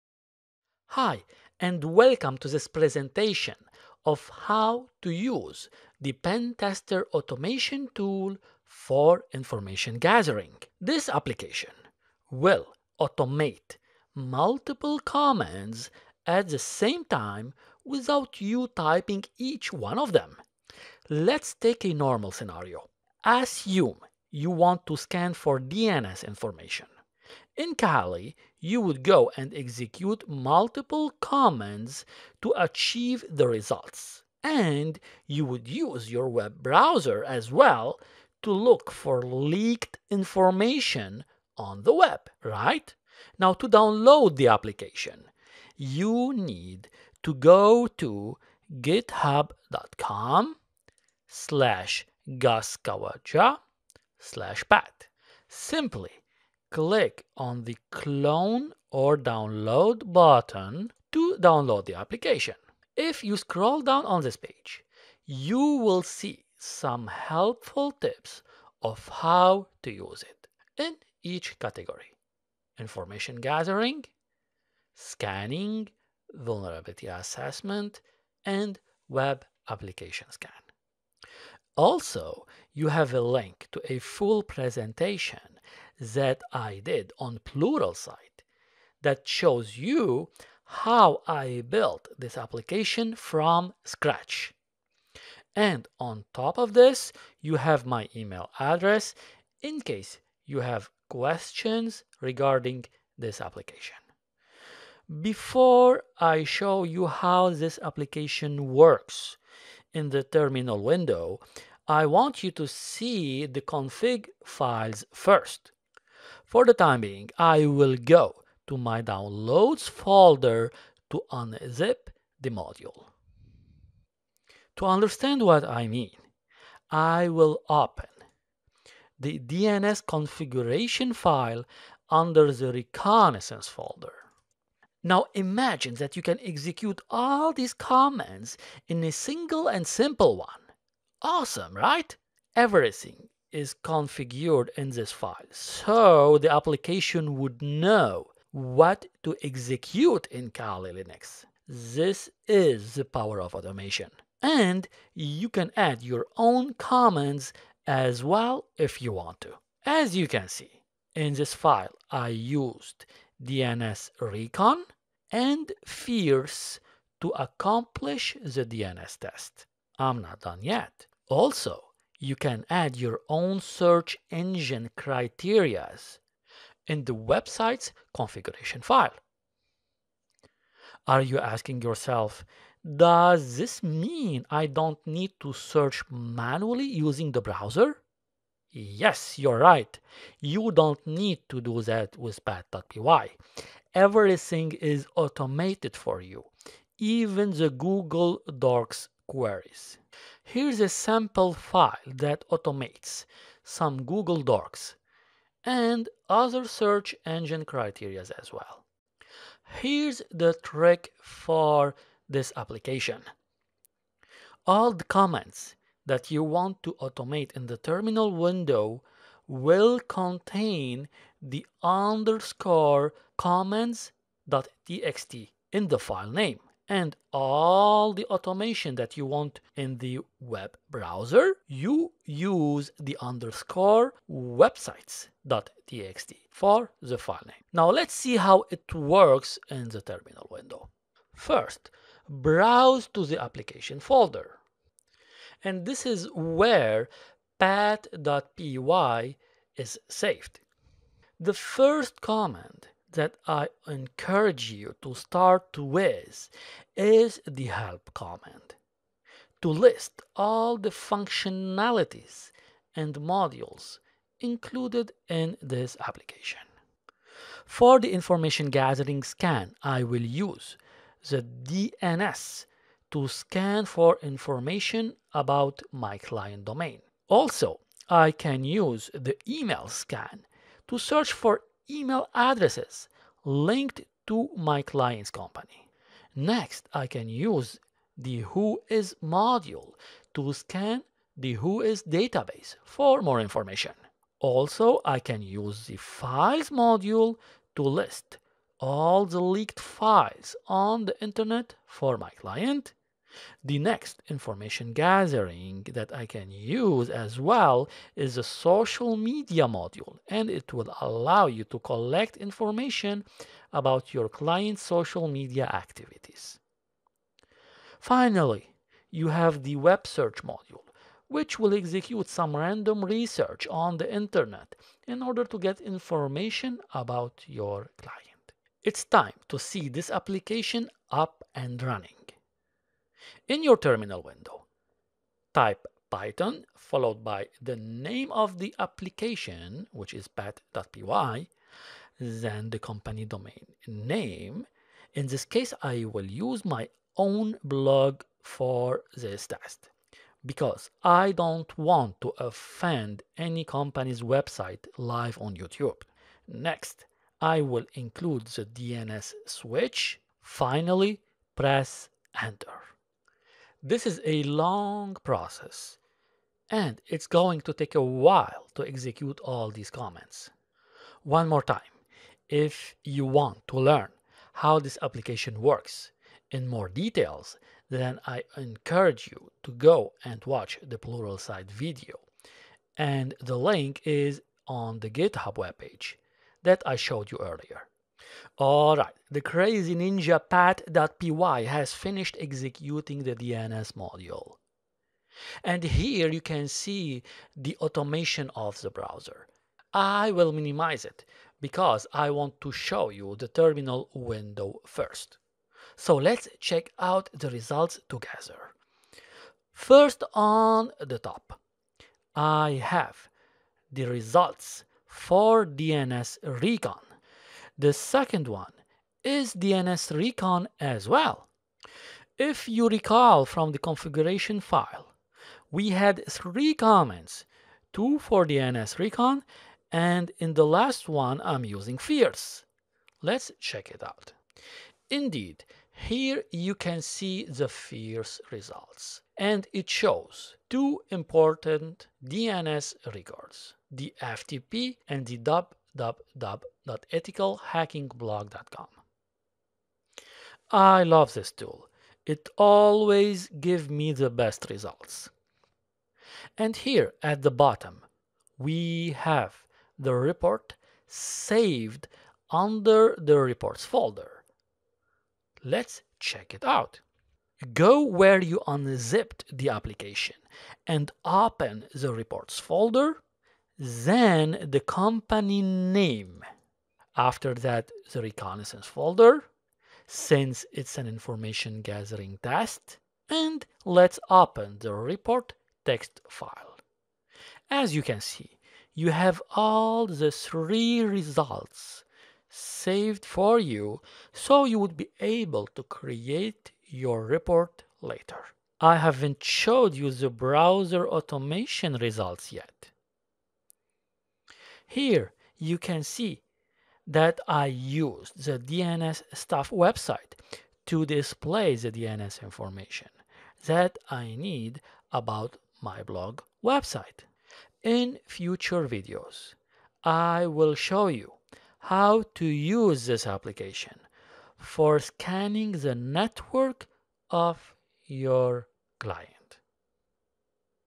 Information Gathering Automation In Kali Linux
Learn how to automate your information gathering tests using pat.py